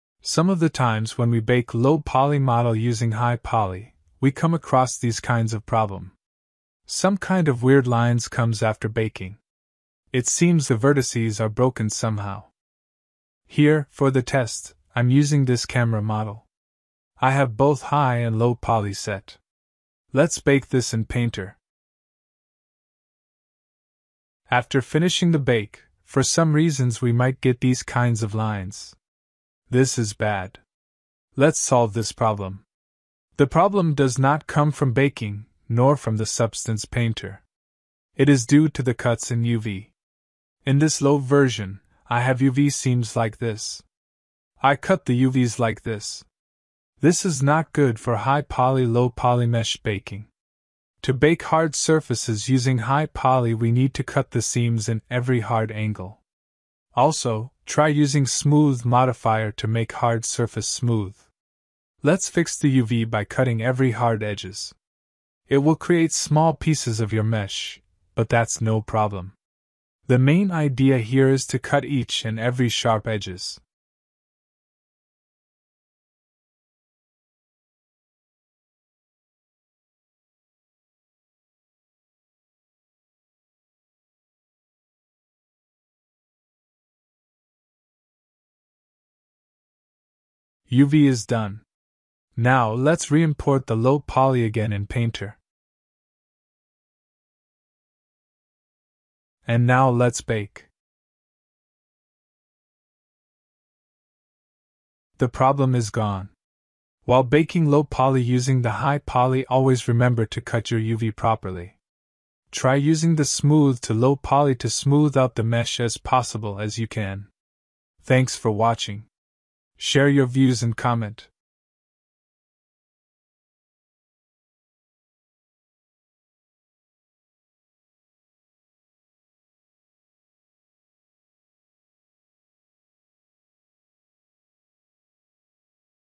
Low-poly baking using high-poly | PROBLEM SOLVED
Are you getting weird cuts like problem in your low poly bake?
Then this video will help you. Let's get started with the video.
Please feel free to drop your suggestions.